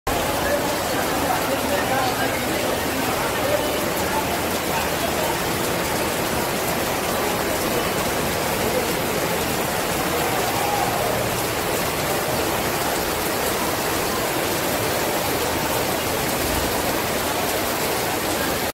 Maltempo, pioggia intensa sull'isola di Ortigia a Siracusa
Dopo una mattinata di sole durante i preparativi dell'Expo e G7 agricoltura